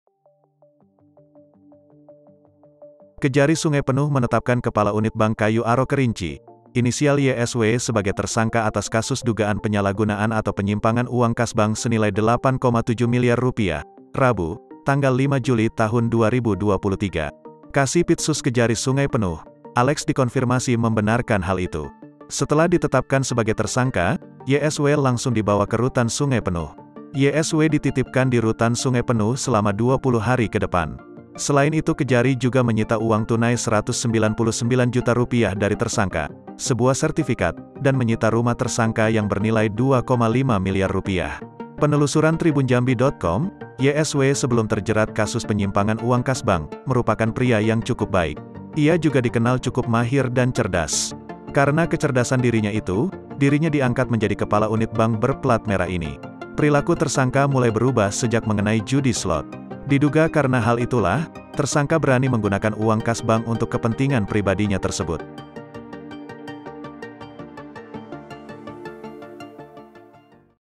Kepala Unit Bank di Kerinci Kuras Uang Kas, Diduga Karena Kecanduan Judi Slot
Kejari Sungai Penuh menetapkan Kepala Unit Bank Kayu Aro Kerinci, inisial YSW sebagai tersangka atas kasus dugaan penyalahgunaan atau penyimpangan uang kas bank senilai Rp8,7 miliar, Rabu (5/7/2023).